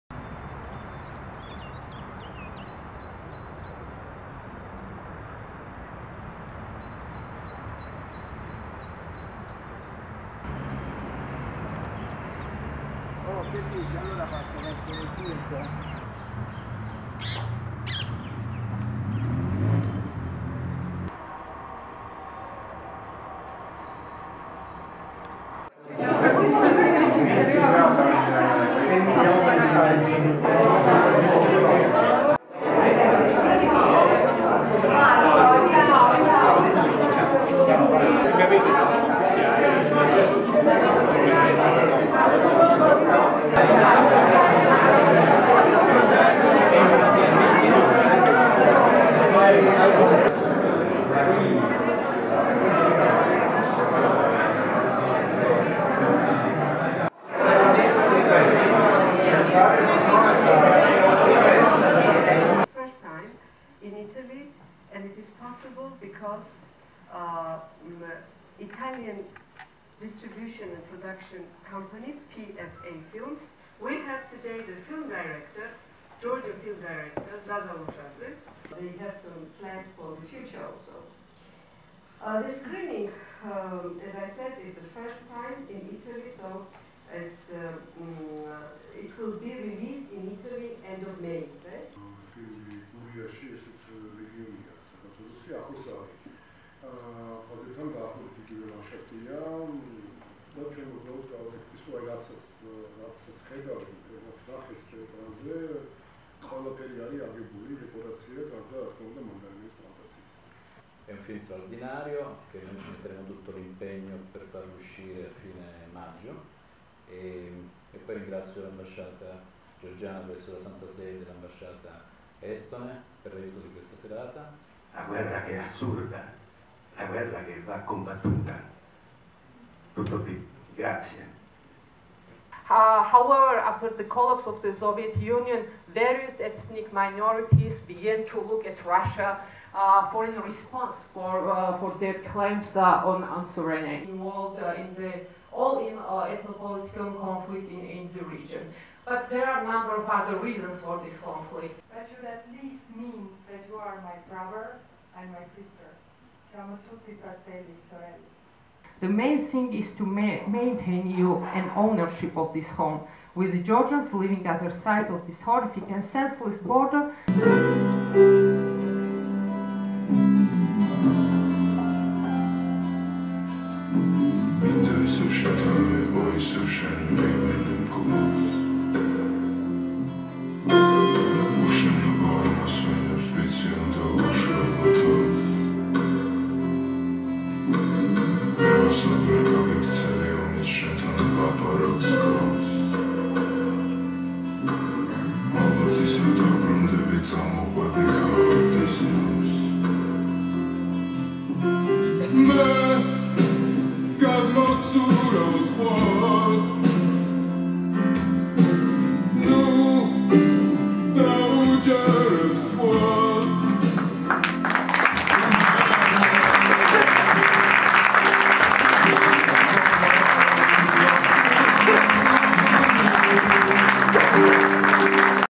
Presentazione del film "Tangerines" a Roma
On 11 March 2016 at "Casa del Cinema" in Rome, Oscar best foreign language film nominee "Tangerines" was presented by the Embassy of Georgia to the Holy See together with the Embassy of Estonia to the Republic of Italy and with PFA Films.

2016 წლის 11 მარტს, რომის კინოს სახლში (Casa del Cinema) წმინდა საყდართან საქართველოს საელჩოს, იტალიის რესპუბლიკაში ესტონეთის საელჩოს და საზოგადოება PFA-ის ორგანიზებით, ასევე ფილმის პროდიუსერების მონაწილეობით, გაიმართა "ოსკარის" ნომინანტი ფილმის "მანდარინები" პრეზენტაცია, რომელიც იტალიურ კინოთეატრებში მიმდინარე წლის მაისში გამოჩნდება.